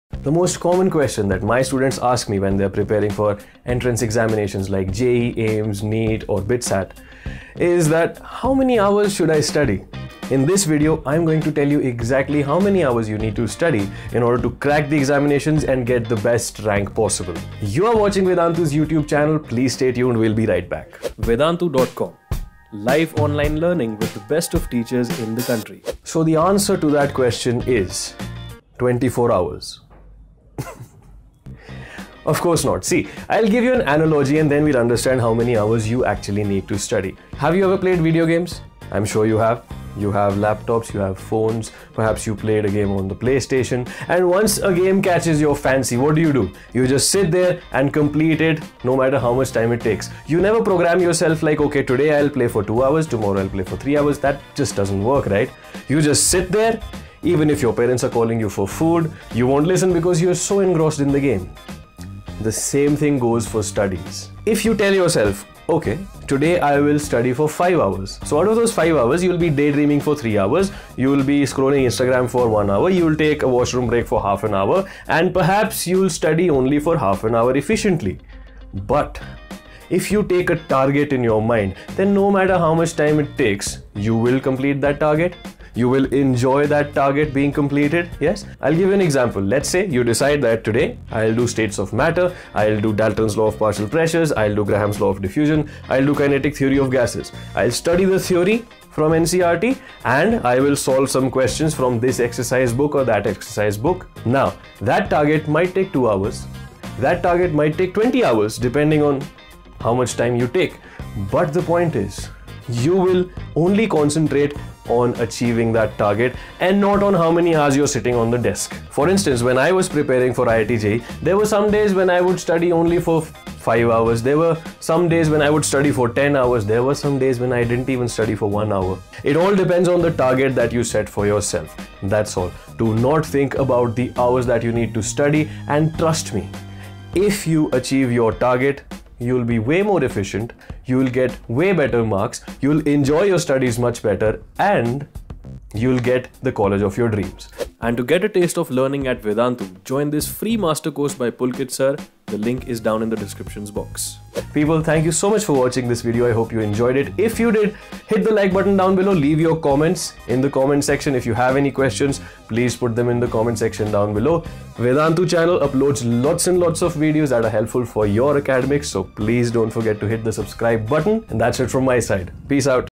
Vedantu How many hours to study to clear JEE | By Pahul Gulati Vedantu Interview
How to study for JEE MAINS | How much to
 study

Hi guys , to ignited ideas.

40 years IIT-JEE LINK -

This is fully unbiassed review and I'm not making any profit from this video .

 cheapbooks
vedantu
jeemain
jeemainpreprationstrategy
iitadvance